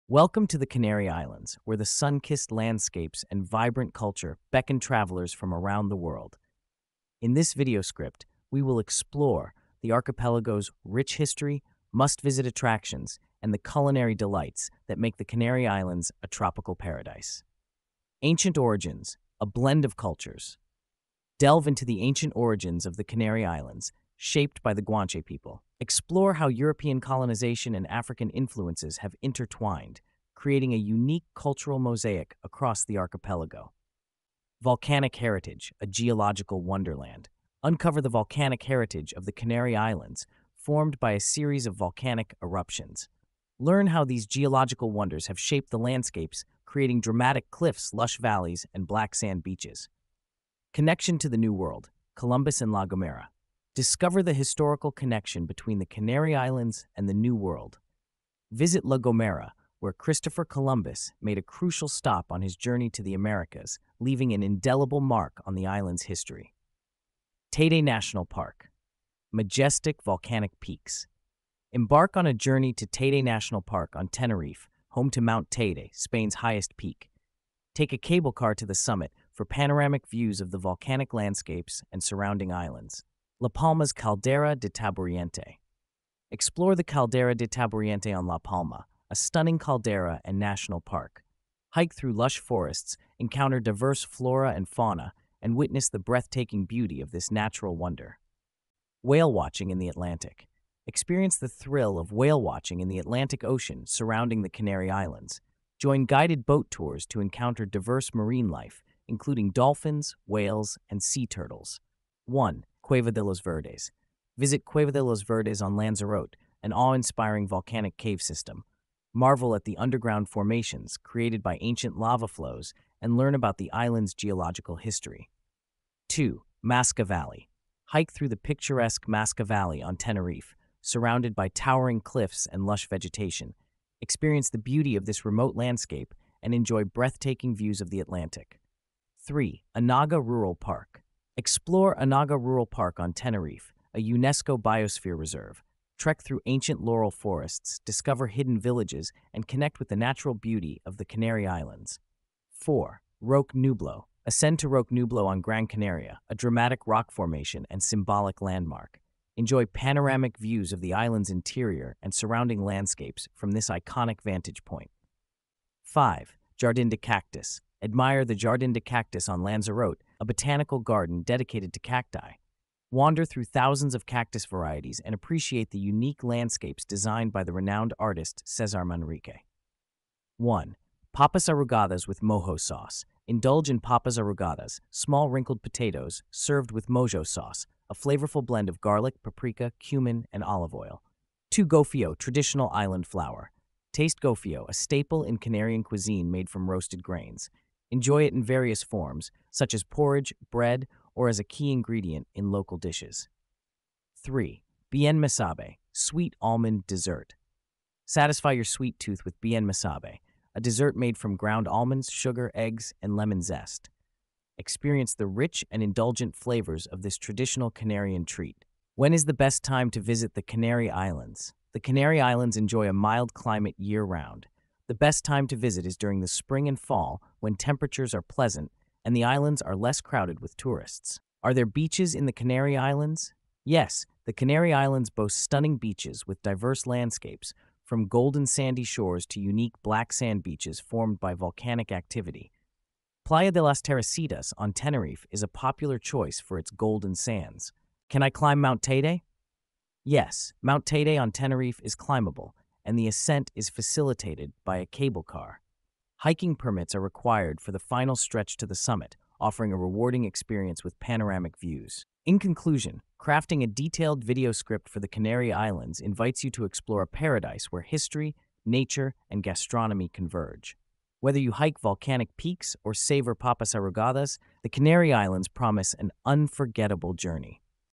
Canary Islands, Spain: A Travel Guide to Attractions, Spanish Delights & FAQ's 💕
Canary Islands: Exploring History, Attractions, and Culinary Wonders

Embark on a sun-soaked adventure through the enchanting Canary Islands as we meticulously craft a video script covering its diverse history, top attractions, and delectable cuisine. Dive into the beauty of this archipelago with our comprehensive guide.

Welcome to the Canary Islands, where the sun-kissed landscapes and vibrant culture beckon travelers from around the world. In this video script, we will explore the archipelago's rich history, must-visit attractions, and the culinary delights that make the Canary Islands a tropical paradise.

Create an Long Detailed Video Script to Include Detailed History
Ancient Origins: A Blend of Cultures
Delve into the ancient origins of the Canary Islands, shaped by the Guanche people. Explore how European colonization and African influences have intertwined, creating a unique cultural mosaic across the archipelago.

Volcanic Heritage: A Geological Wonderland
Uncover the volcanic heritage of the Canary Islands, formed by a series of volcanic eruptions. Learn how these geological wonders have shaped the landscapes, creating dramatic cliffs, lush valleys, and black sand beaches.

Connection to the New World: Columbus and La Gomera
Discover the historical connection between the Canary Islands and the New World. Visit La Gomera, where Christopher Columbus made a crucial stop on his journey to the Americas, leaving an indelible mark on the island's history.

Things to Do While Visiting in the Canary Islands
Teide National Park: Majestic Volcanic Peaks
Embark on a journey to Teide National Park on Tenerife, home to Mount Teide, Spain's highest peak. Take a cable car to the summit for panoramic views of the volcanic landscapes and surrounding islands.

La Palma's Caldera de Taburiente
Explore the Caldera de Taburiente on La Palma, a stunning caldera and national park. Hike through lush forests, encounter diverse flora and fauna, and witness the breathtaking beauty of this natural wonder.

Whale Watching in the Atlantic
Experience the thrill of whale watching in the Atlantic Ocean surrounding the Canary Islands. Join guided boat tours to encounter diverse marine life, including dolphins, whales, and sea turtles.

5 Attractions to Visit in the Canary Islands
1. Cueva de los Verdes
Visit Cueva de los Verdes on Lanzarote, an awe-inspiring volcanic cave system. Marvel at the underground formations, created by ancient lava flows, and learn about the island's geological history.

2. Masca Valley
Hike through the picturesque Masca Valley on Tenerife, surrounded by towering cliffs and lush vegetation. Experience the beauty of this remote landscape and enjoy breathtaking views of the Atlantic.

3. Anaga Rural Park
Explore Anaga Rural Park on Tenerife, a UNESCO Biosphere Reserve. Trek through ancient laurel forests, discover hidden villages, and connect with the natural beauty of the Canary Islands.

4. Roque Nublo
Ascend to Roque Nublo on Gran Canaria, a dramatic rock formation and symbolic landmark. Enjoy panoramic views of the island's interior and surrounding landscapes from this iconic vantage point.

5. Jardin de Cactus
Admire the Jardin de Cactus on Lanzarote, a botanical garden dedicated to cacti. Wander through thousands of cactus varieties and appreciate the unique landscapes designed by the renowned artist César Manrique.

5 Most Exciting Popular Foods to Eat in the Canary Islands
1. Papas Arrugadas with Mojo Sauce
Indulge in Papas Arrugadas, small wrinkled potatoes, served with Mojo Sauce—a flavorful blend of garlic, paprika, cumin, and olive oil.

2. Gofio: Traditional Island Flour
Taste Gofio, a staple in Canarian cuisine made from roasted grains. Enjoy it in various forms, such as porridge, bread, or as a key ingredient in local dishes.

3. Bienmesabe: Sweet Almond Dessert
Satisfy your sweet tooth with Bienmesabe, a dessert made from ground almonds, sugar, eggs, and lemon zest. Experience the rich and indulgent flavors of this traditional Canarian treat.

Create an Long Detailed Video Script: 3 FAQ's
When is the Best Time to Visit the Canary Islands?
The Canary Islands enjoy a mild climate year-round. The best time to visit is during the spring and fall when temperatures are pleasant, and the islands are less crowded with tourists.

Are There Beaches in the Canary Islands?
Yes, the Canary Islands boast stunning beaches with diverse landscapes, from golden sandy shores to unique black sand beaches formed by volcanic activity. Playa de las Teresitas on Tenerife is a popular choice for its golden sands.

Can I Climb Mount Teide?
Yes, Mount Teide on Tenerife is climbable, and the ascent is facilitated by a cable car. Hiking permits are required for the final stretch to the summit, offering a rewarding experience with panoramic views.